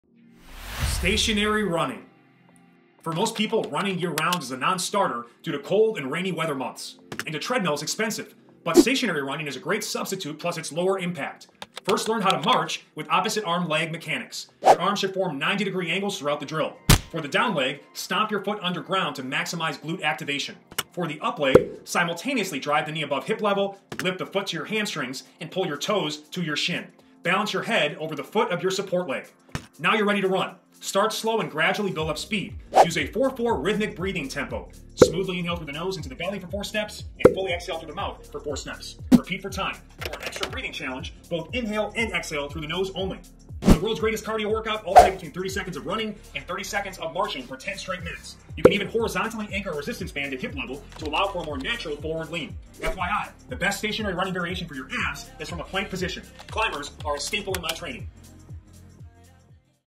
🏃‍♂️ How To Run at Home Without a Treadmill
Learn How To Run at Home Without a Treadmill from BJ Gaddour, former Men's Health fitness director!

For most people, running year-round is a non-starter due to cold or rainy weather months. And a treadmill is expensive. But Stationary Running is a great substitute plus it’s lower-impact.

First learn how to March with opposite arm-leg mechanics. Your arms should form 90-degree angles throughout the drill. For the down leg, stomp your foot underground to maximize glute activation. For the up leg, simultaneously drive the knee above hip level, lift the foot to your hamstrings & pull your toes to your shin. Balance your head over the foot of your support leg.

Now you’re ready to run. Start slow and gradually build up speed. Use a 4/4 Rhythmic Breathing Tempo: smoothly inhale through the nose into the belly for 4 steps and fully exhale through the mouth for 4 steps. Repeat for time. For an extra breathing challenge, both inhale & exhale through the nose only.

For the world’s greatest cardio workout, alternate between 30 seconds of running & 30 second of marching for 10 straight minutes. You can even horizontally anchor a resistance band at hip-level to allow for a more natural forward lean. FYI- the best stationary running variation for your abs is from a plank-position. Climbers are a staple in my training.

In this video, I'm going to show you how to run at home without a treadmill! This SHORT video will teach you the basics of how to run at home, using only your own body and clothes!

If you're looking for a way to get a cardio workout without using a treadmill, then this video is for you! By following the tips in this video, you'll be able to run efficiently and enjoy the outdoors without having to go to the gym!